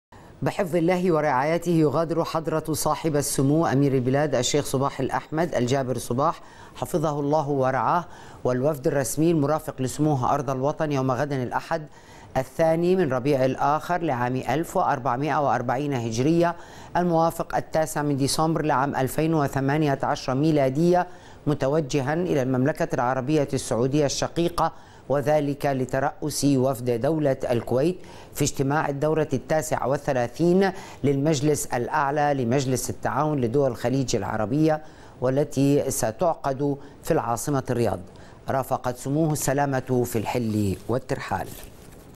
سمو أمير البلاد يتوجه غدا إلى السعودية لترؤس وفد الكويت بإجتماع المجلس الأعلى لمجلس التعاون
سمو أمير البلاد يتوجه غدا إلى السعودية لترؤس وفد الكويت بالاجتماع الـ 39 للمجلس الأعلى لمجلس التعاون الخليجي